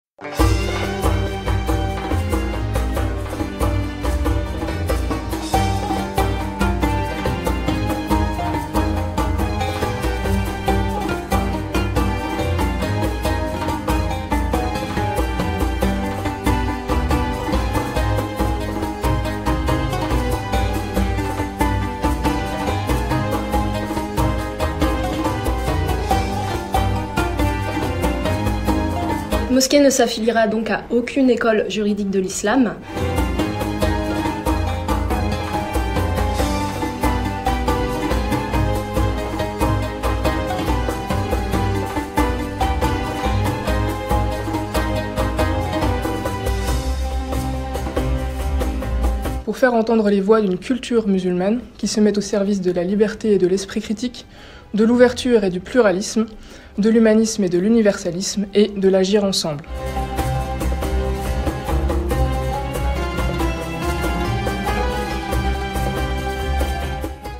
Ces deux femmes révolutionnent l'islam en France ! 😮👩🏻🕌
Ces deux femmes révolutionnent l'islam en France ! Anne-Sophie et Eva ont dirigé une prière dans une mosquée à Paris ... 😮👩🏻🕌